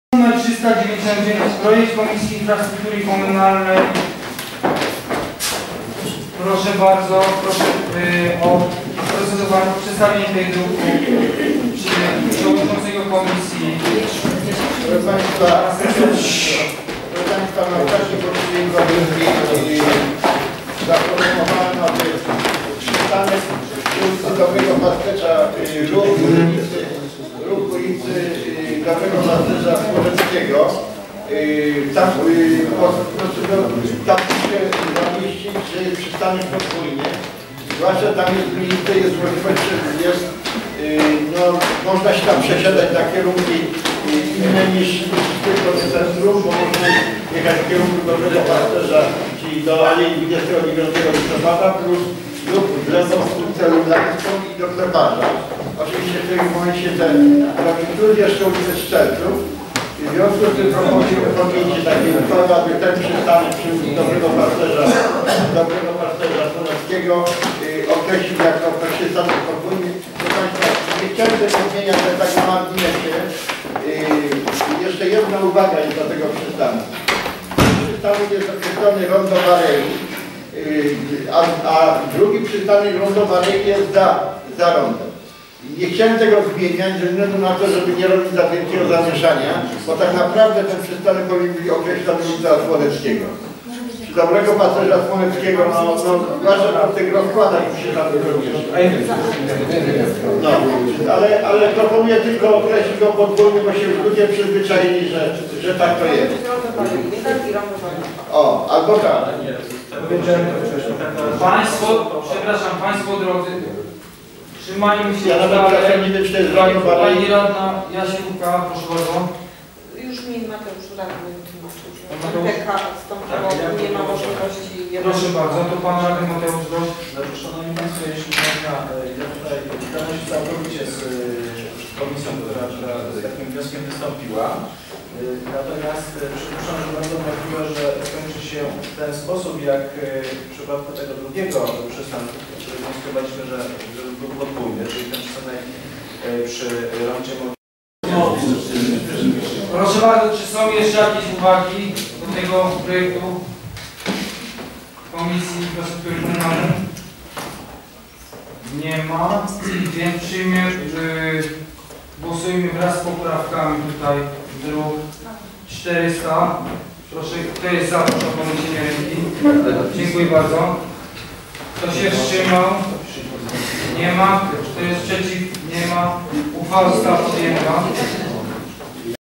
RD_III_28-05-2013_18 wprowadzenie podwójnego przystanku ul. Dobrego Pasterza
Nagranie z XXXIII sesji Rady Dzielnicy III Prądnik Czerwony z dnia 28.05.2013 r.-cz.18- omawiane tematy i głosowania:

Uchwała nr XXXIII/385/2013 w sprawie utworzenia przystanku podwójnego „Rondo Barei" przy skrzyżowaniu ulic Słoneckiego -- Dobrego Pasterza.